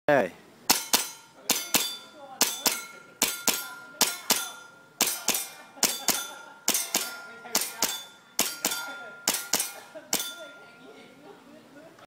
Little Brother Beer (The New THASA)
I was blessed to have a chance to meet a brother from my old school. He has the same sporting background like I do in rugby and golf.

Now 2 questions here.
1. Would you believe that he was a hardcore weaver style that it took almost an hour and a half to adjust to isosceles. ???
2. Would you believe that this is only his 2nd time in shooting airsoft practical shooting style...???

You should. ^^